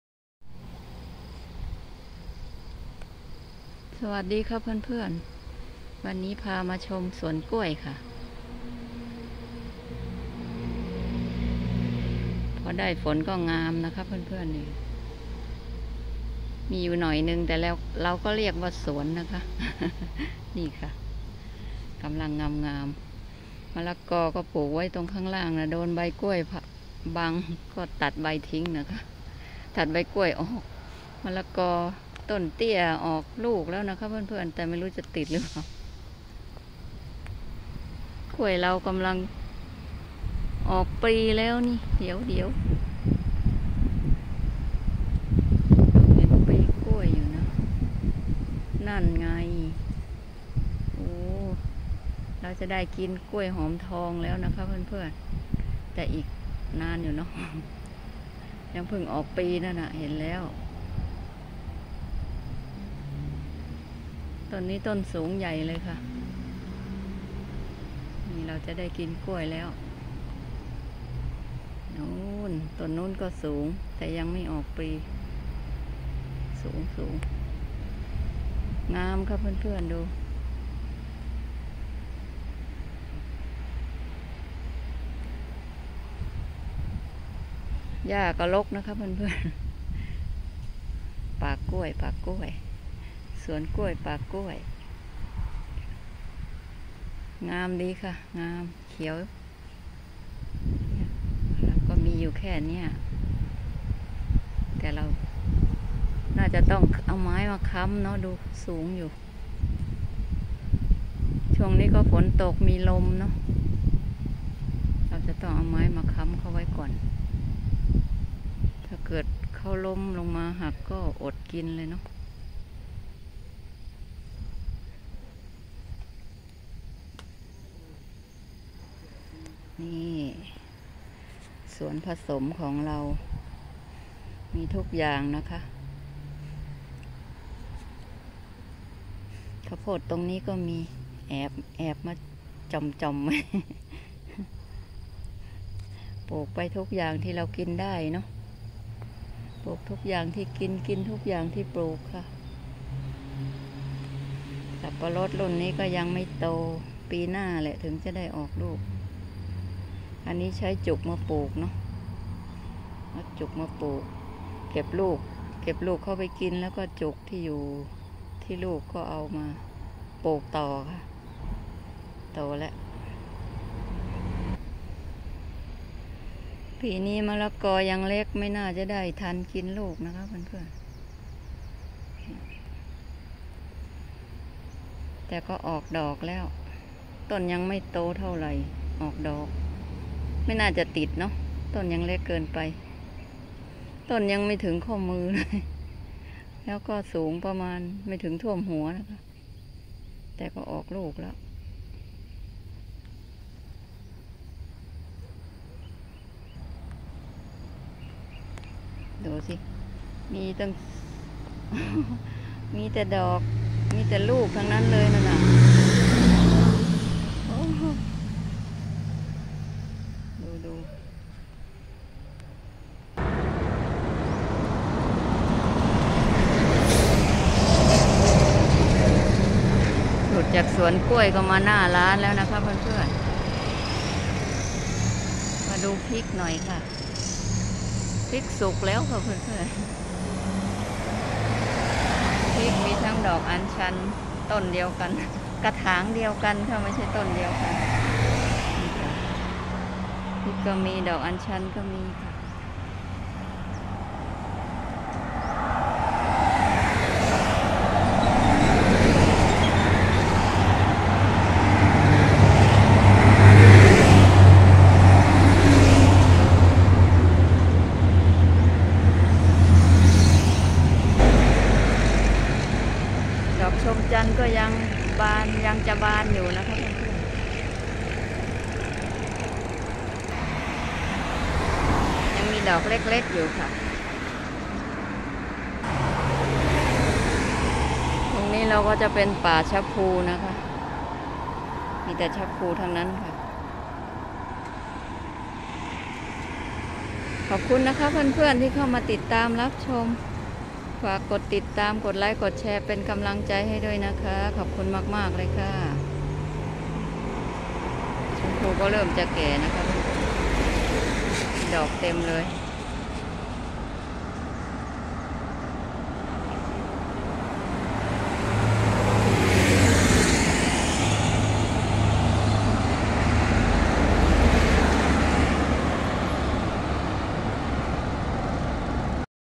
พาชมพืชสวนผสม ผักสวนครัว ปลูกผักกินเอง จินตนาปลูกผัก Chintana Garden
วันนี้พามาดูกล้วยหอมทองและพืชผักสวนผสมค่ะ ปลูกกินเองไม่ต้องซื้อประหยัดค่าใช้จ่ายค่ะ จินตนาปลูกผัก Chintana Garden